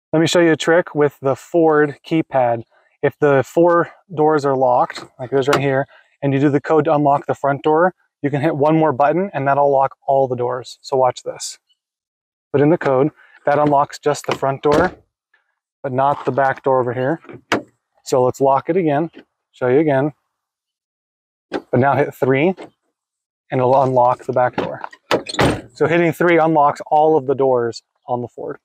Unlock the other doors on your Ford with the keypad
Did you know that you can use the keypad on your Ford to do more than just unlock the driver's door? After you punch in your code to unlock the door, immediately hit 3 and it will unlock all the doors for the whole vehicle.